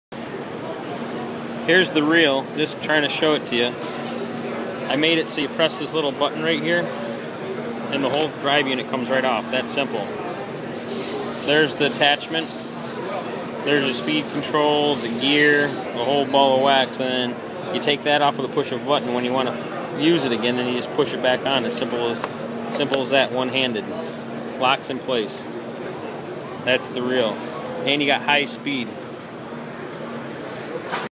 Manual hooker
new hooker electric manual reel. the electric part of the reel easily comes off at a press of a button and gives you full hi speed for fighting a fish manually.